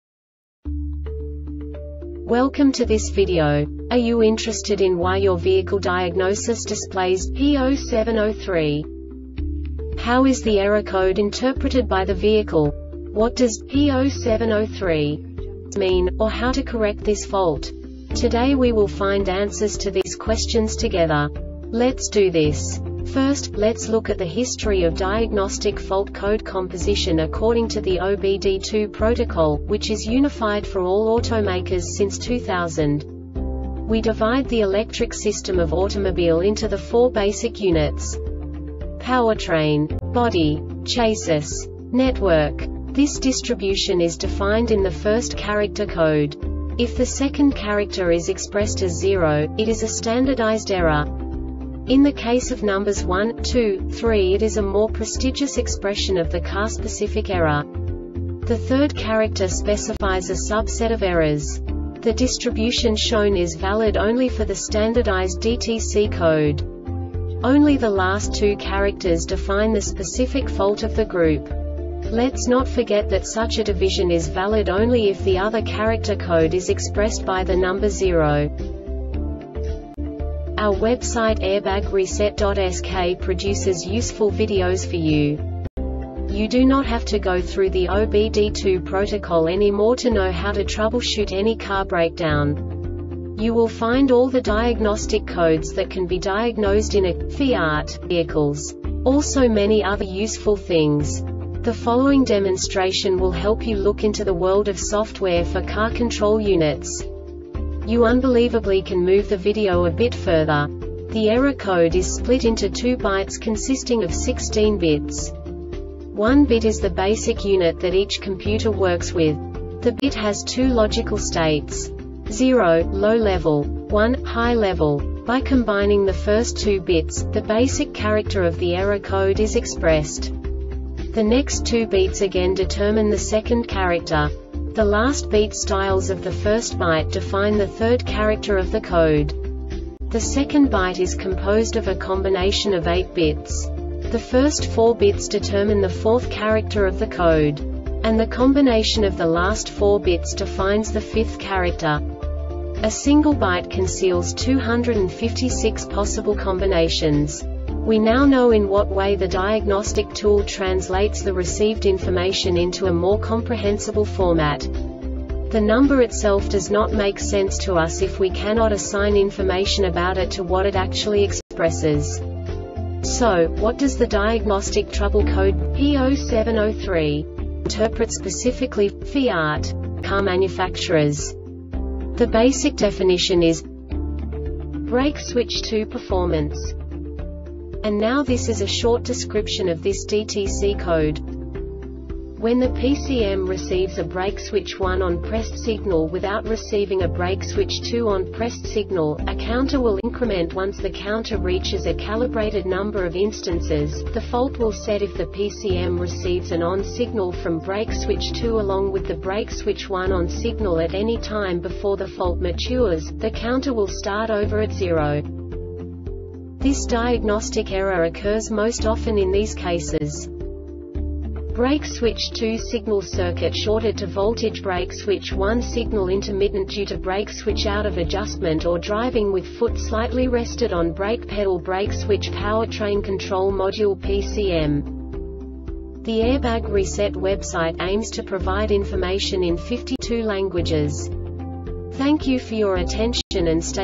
DTC Fiat P0703 Short Explanation
Contents:
0:21 Basic DTC analysis according to OBD2 protocol standard.
1:48 Insight into programming
2:58 A diagnostically understandable description of the Diagnostic Trouble Code
3:05 Basic error definition

Make:
FIAT
Code:
P0703
Definition:
Brake Switch 2 Performance
Description:
When the PCM receives a Brake Switch 1 ON (Pressed) signal without receiving a Brake Switch 2 ON (Pressed) signal, a counter will increment. Once the counter reaches a calibrated number of instances, the fault will set. If the PCM receives an ON signal from Brake Switch 2 along with the Brake Switch 1 ON signal at any time before the fault matures, the counter will start over at zero.
Cause:
Brake Switch 2 Signal Circuit Shorted To Voltage
Brake Switch 1 Signal Intermittent Due To Brake Switch Out Of Adjustment Or Driving With Foot Slightly Rested On Brake Pedal
Brake Switch